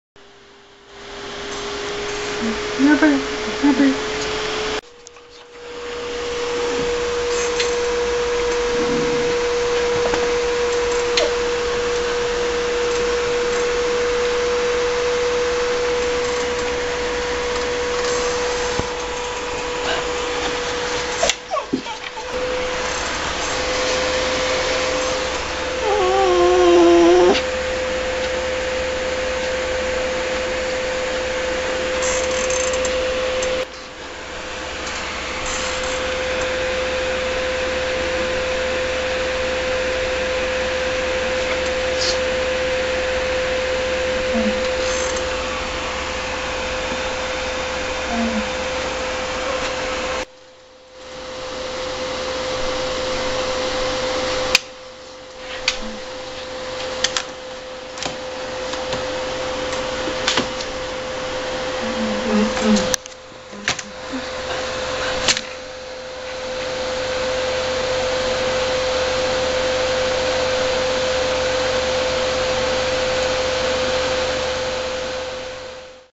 Dog Watching Video
My dog, Zorro is watching a video on the computer about a strange relationship between dogs and polar bears!